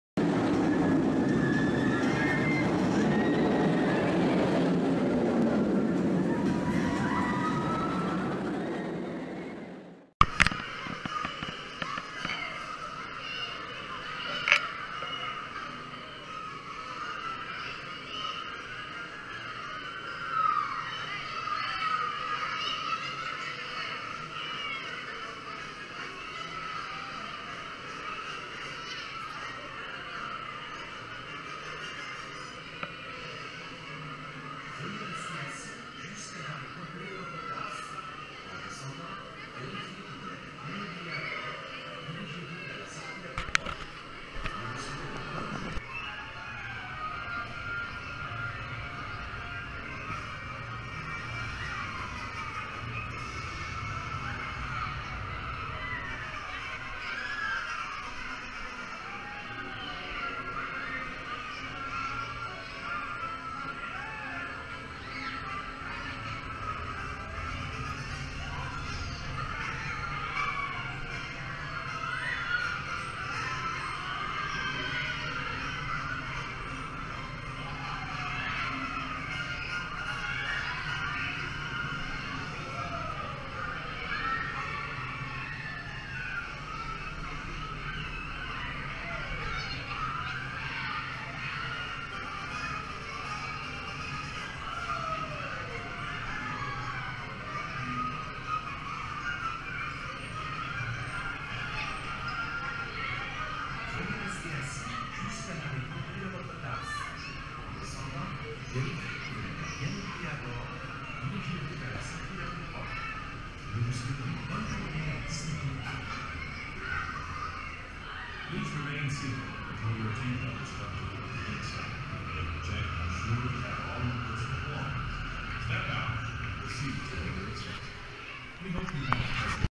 Mad Hatter's Tea Cups Disneyland Paris 2016
Mad Hatter's Tea Cups of Alice in Wonderland Disneyland Paris 2016 onride complet attraction
Les Tasses du Chapelier de Fou de alice au pays des merveilles  Disneyland Paris 2016 Attraction complète onride